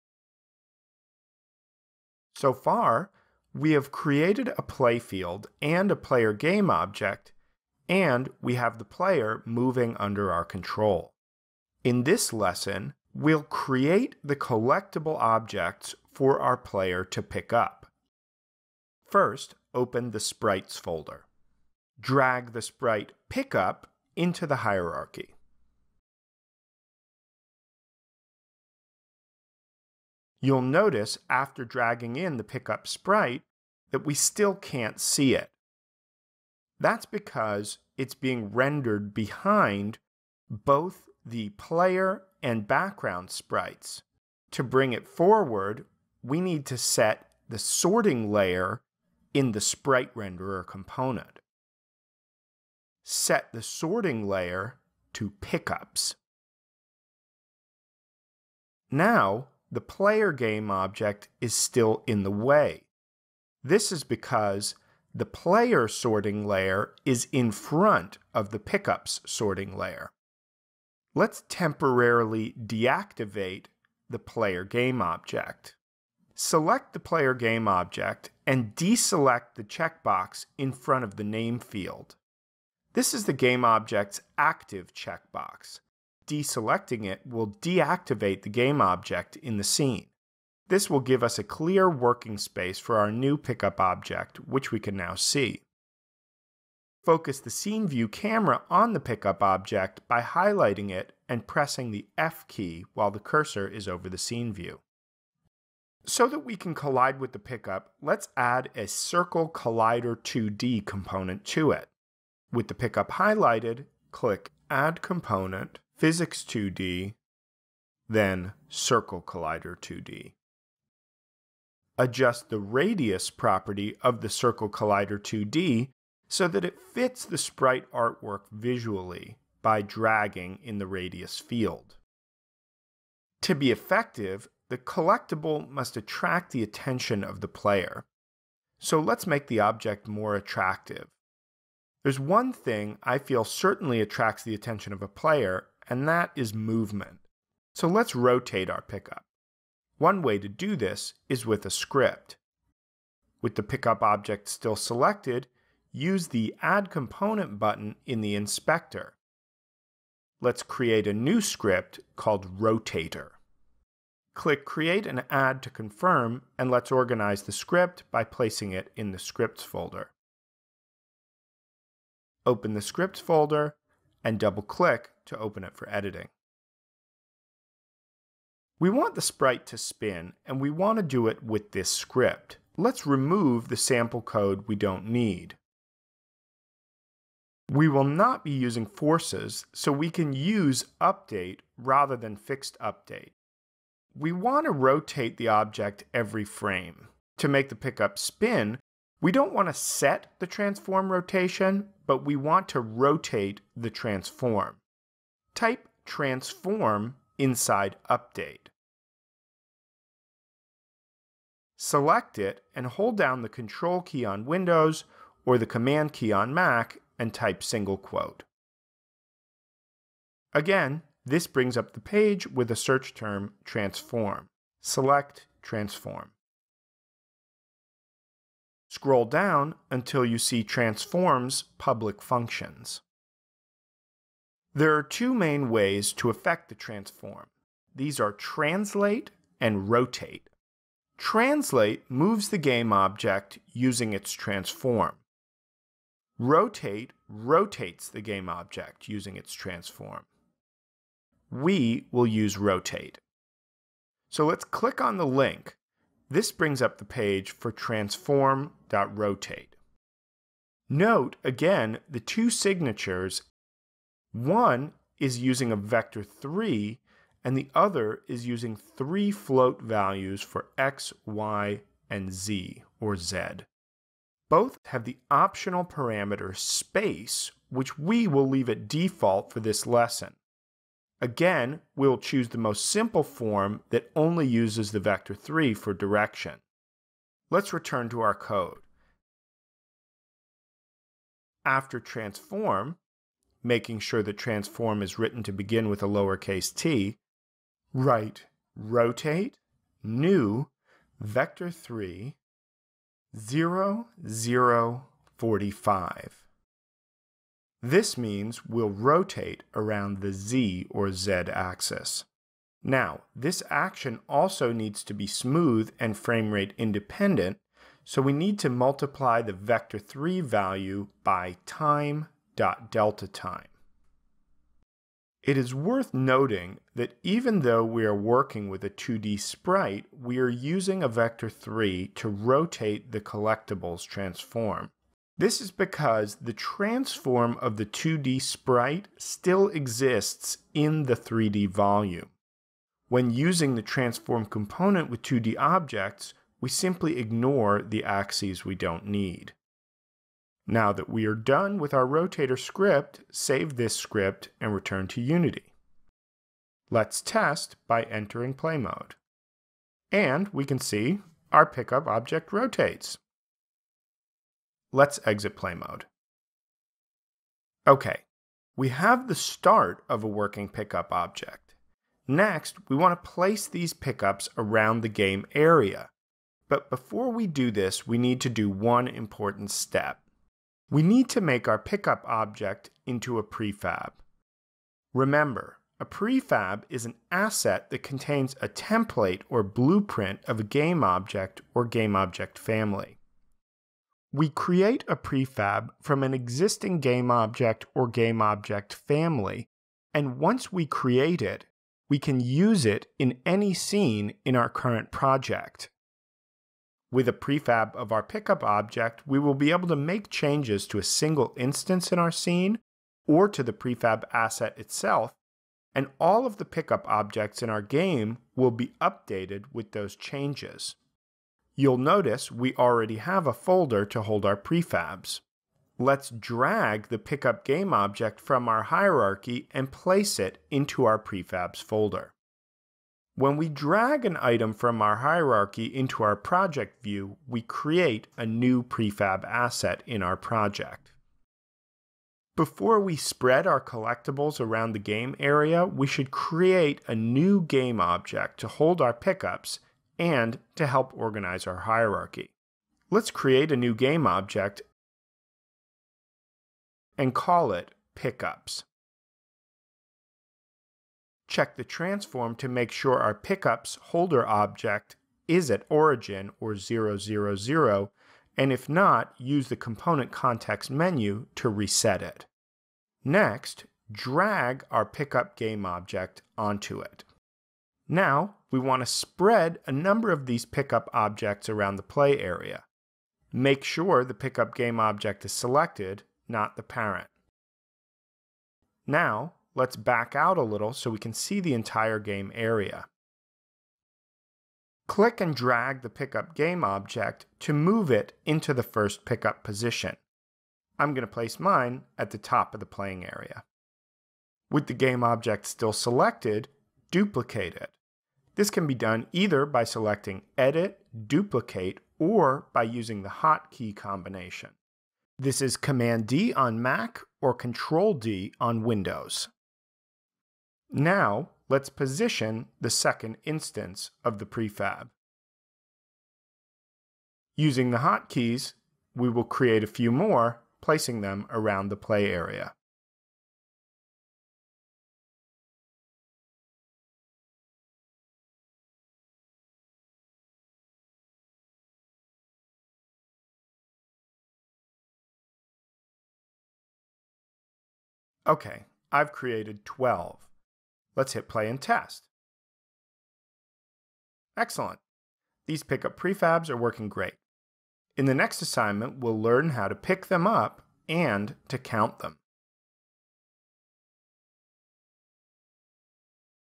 Beginner 2D UFO Game 6 of 9: Creating Collectable Objects - Unity Official Tutorials
Questions?

In this assignment we'll add collectable objects to our 2D UFO game in progress. We'll use Prefabs, 2D trigger colliders to do this, and add a one line C# script to make the collectable objects rotate.